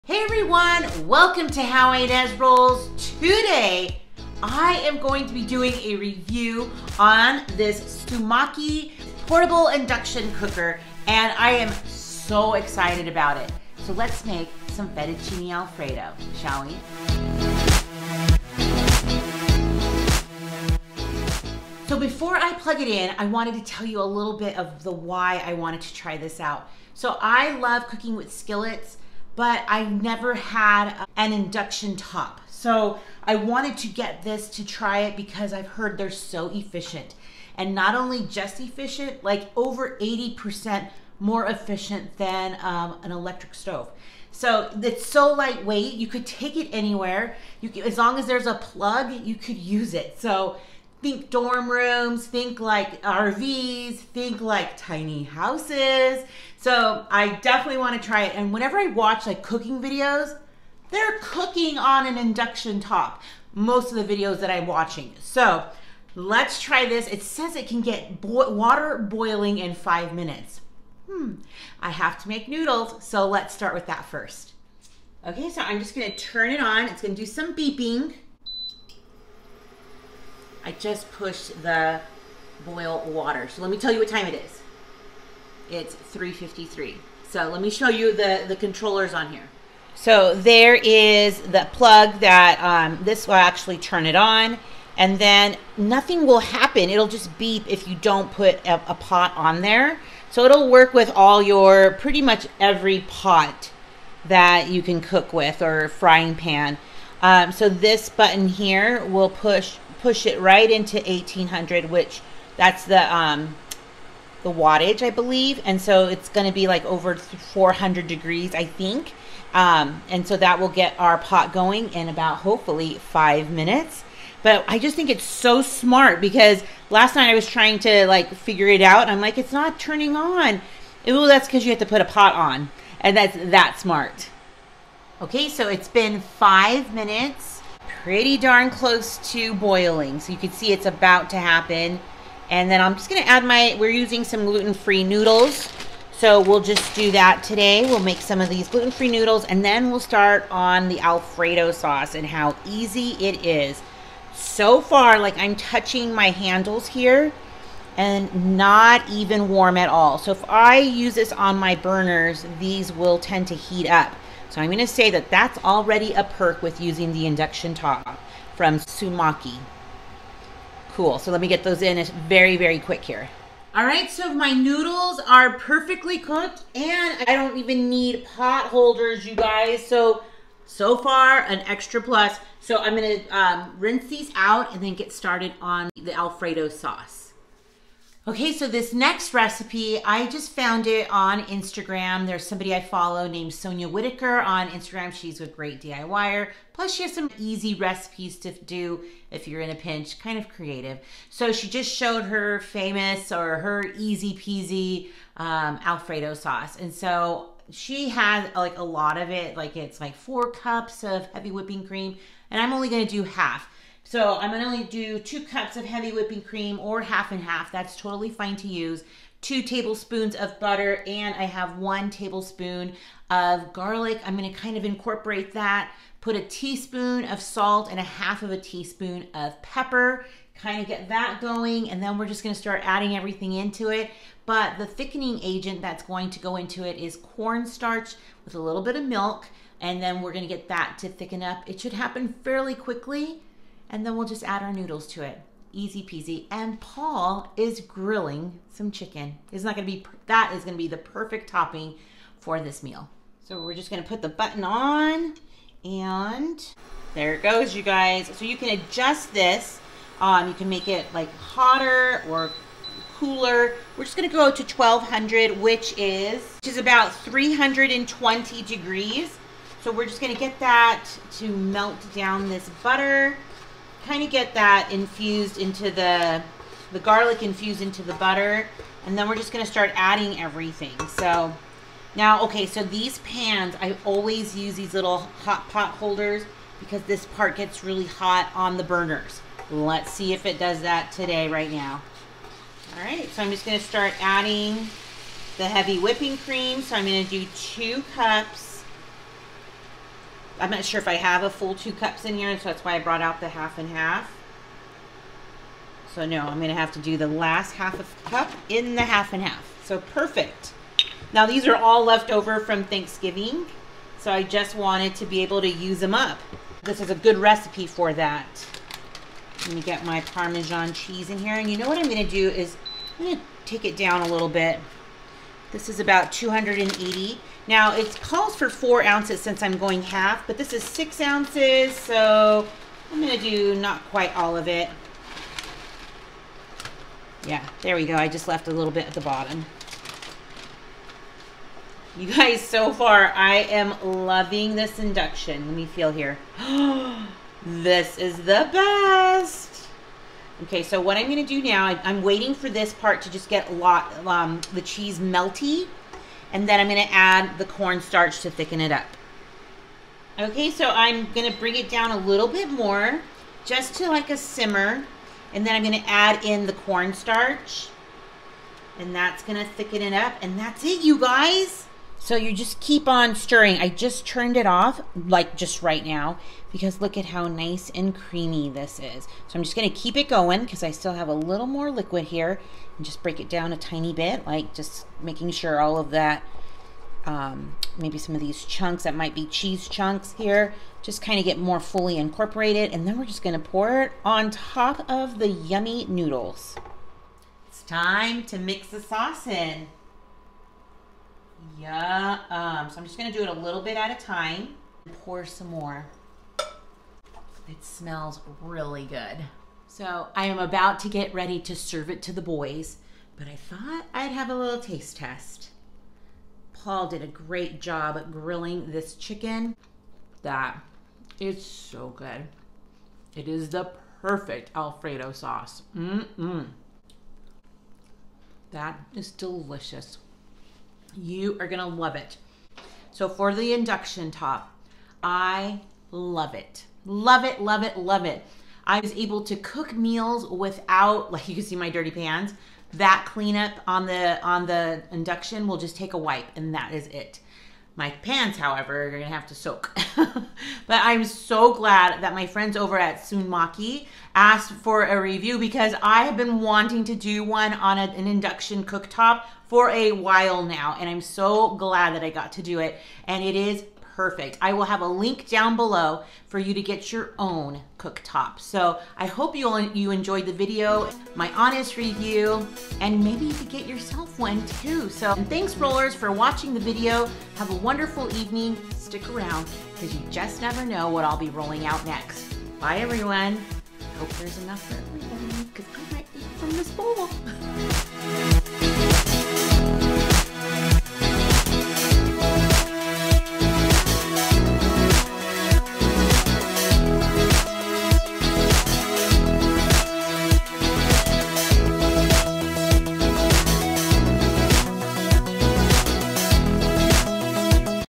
How to Make Easy Chicken Alfredo \ Reviewing Sunmaki’s Induction Cooker
Hey Rollers! I have an awesome review for you. It's SUNMAKI'S INDUCTION COOKER!! It's lightweight, portable & highly efficient. It's so easy to use & if you are needing more cooking space … here's your answer!  Check out how I make delicious Chicken Alfredo with the Induction Cooker!

Here's the recipe to the Chicken Alfredo (credit to: Sonia Whittaker on IG) Remember I did half this recipe!
Alfredo Sauce
1 quart of Heavy Cream or Half/Half
2T. Garlic
4T. Butter
3T. Salt/Pepper Mix
7T. Cornstarch
8oz. Parm Cheese

Heat butter & garlic, salt/pepper mix...add cream & cheese until the cheese is melting. Mix the cornstarch with equal parts milk, then add to the cheese sauce. Keep stirring until thick. Pour over noodles. Top with Chicken!